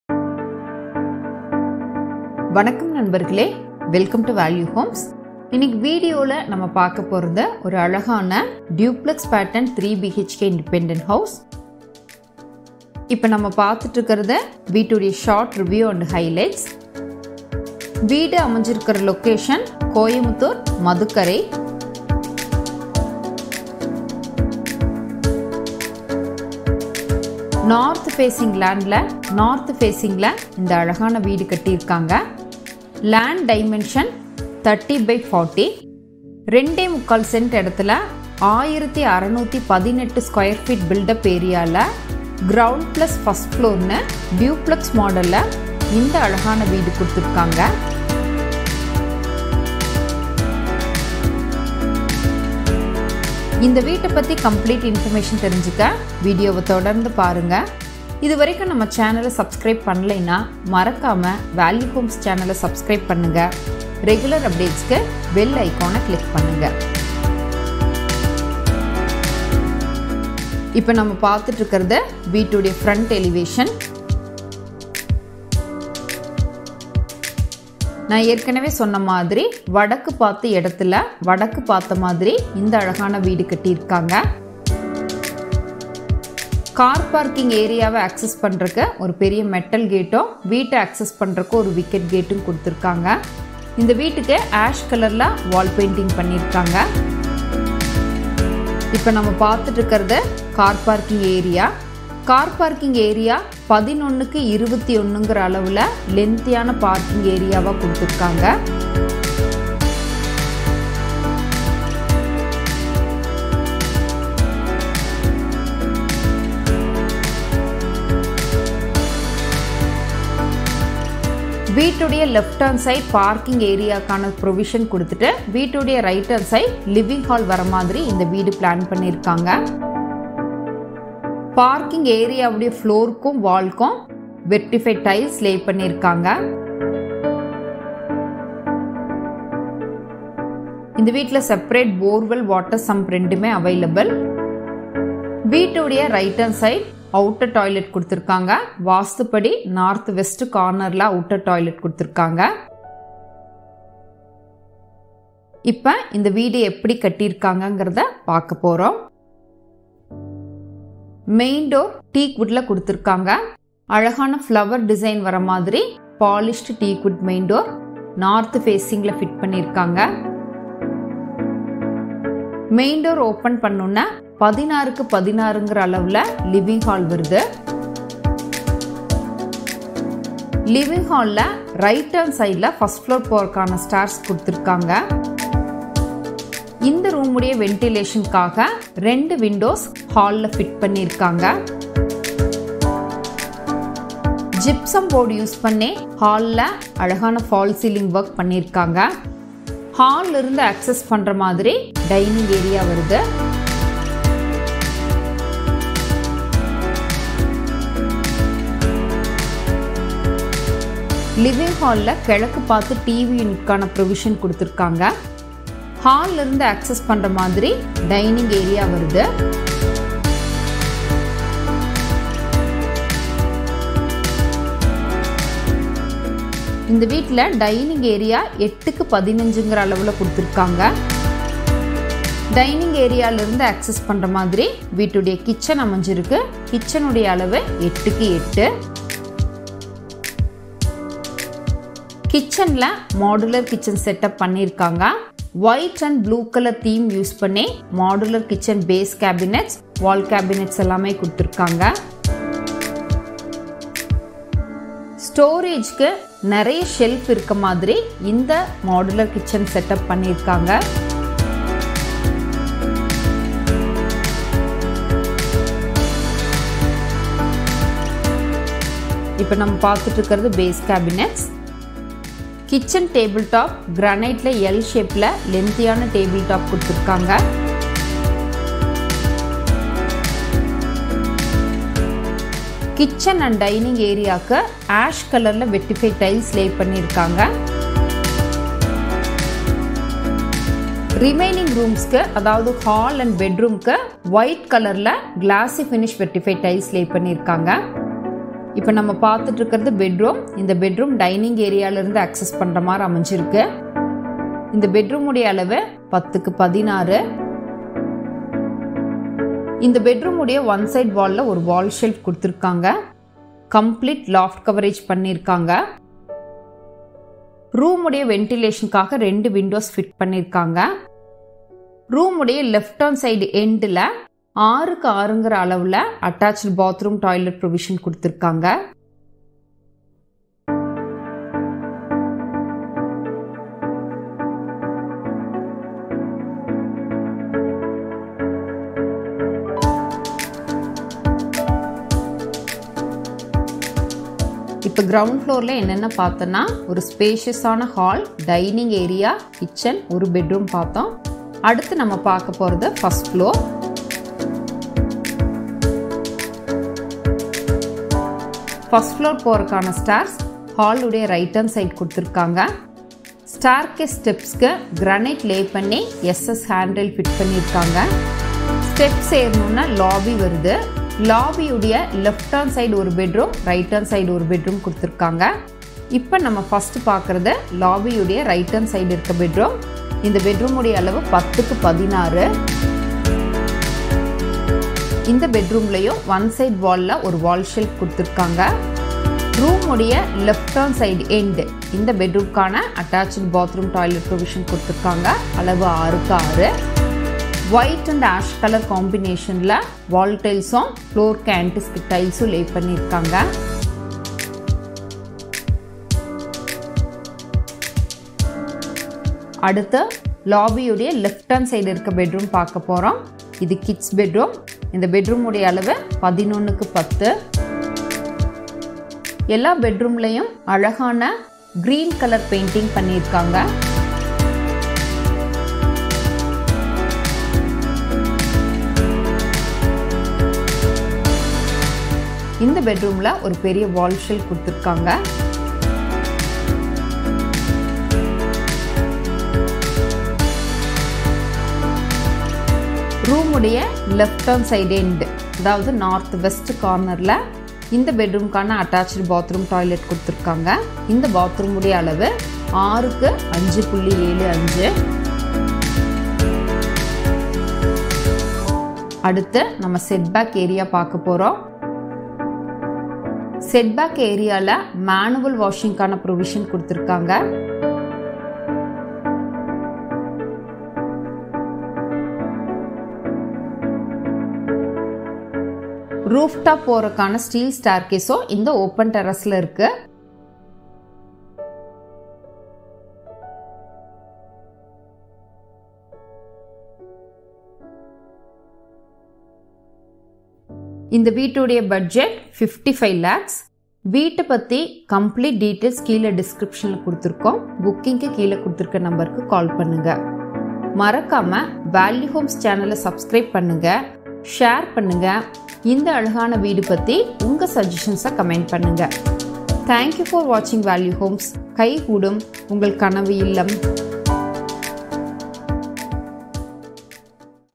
55 லட்சத்தில் கட்டப்பட்ட அழகிய 3BHK வீடு | 3BHK Spacious House in Madukkarai | VH299
ValueHomes கை கூடும் கனவு இல்லம் !

Property ID: VH299

1600 Sqft ல் அழகான 3BHK வீடு | Double Story 3BHK Independent House for Sale hometour | VH299

PROPERTY DETAILS
Site: North Facing
Main Door: North Facing
Land Size: 2.75 cent
Land Dimension: 30 x 40
Builtup Area: 1600 sqft
Location: Madukkarai Market Coimbatore.

House Total Price Including Land is 55 Lakhs (negotiable)
Land Value 8 Lakhs/cent

GROUND FLOOR:
Car Parking: 11’ x 21’
Living Hall: 16’ x 16
Kitchen: 8’ x 8’
Dining: 8 x 14.5'
First Bedroom: 10’ x 16’
Attached Bathroom: 6’ x 6’
Outer Toilet: 3.10' x 4'

FIRST FLOOR:
Second Bedroom: 10’x 16’
Third Bedroom: 11’ x 10’
Attached Bathroom: 5.75’ x 6’

Near to Schools, Colleges, Hospitals, ATMs, Bank, Super Markets and Petrol Bunks etc.,

Value Homes Channel: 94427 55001

/// House for Sale in Coimbatore List ///